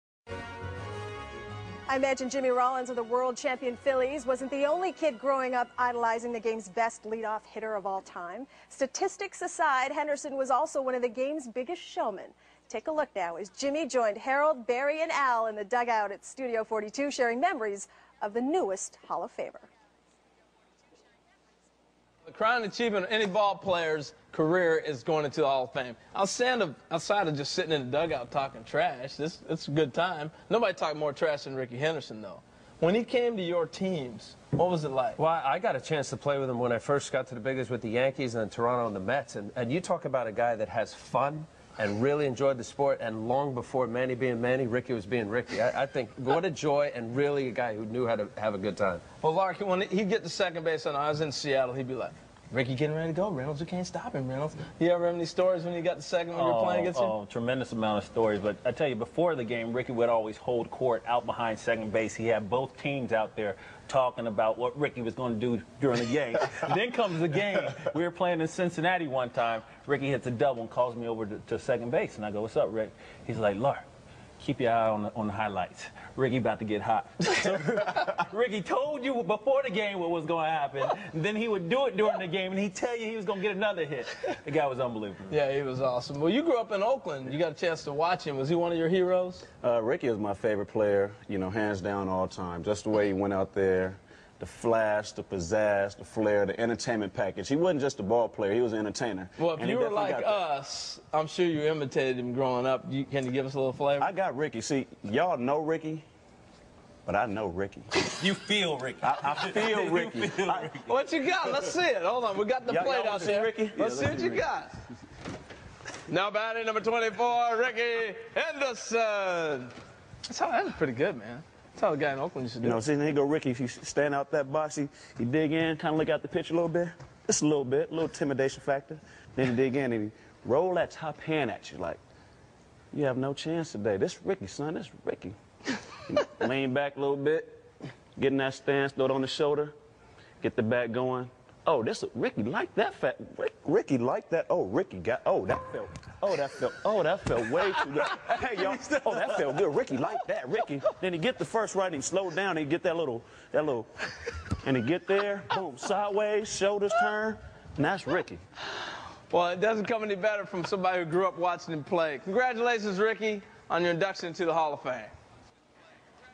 Jimmy Rollins on MLBTV Talking About Rickey Henderson
Jimmy Rollins, Harold Reynolds, Al Leiter and Barry Larkin talking about Rickey Henderson